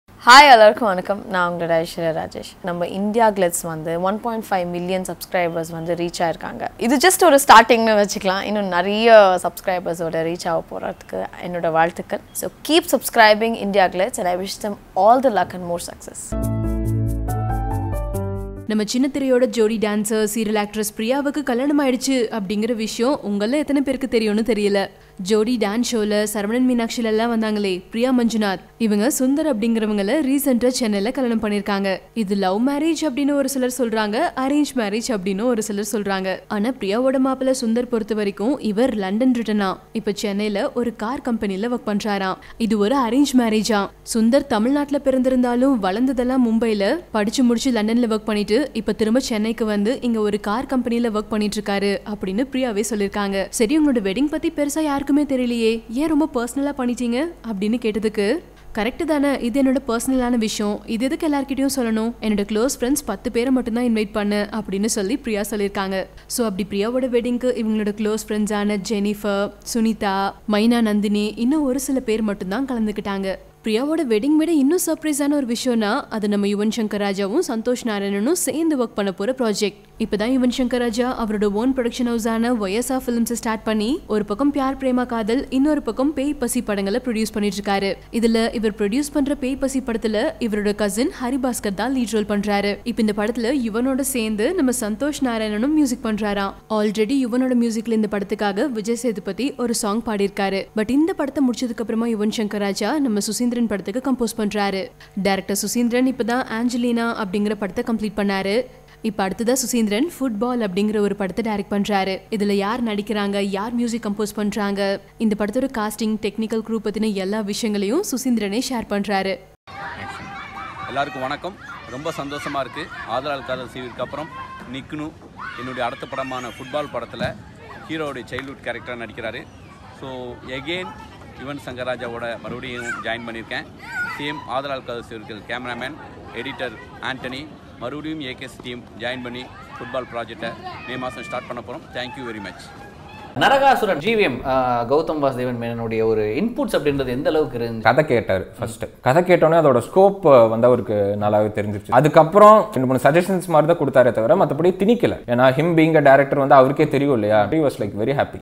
Saravanan Meenatchi Priya got Hitched in a Secret affair | Latest News
Priya, Saravanan Meenatchi fame, got Hitched in a Hindu ceremony recently. It was a private family affair.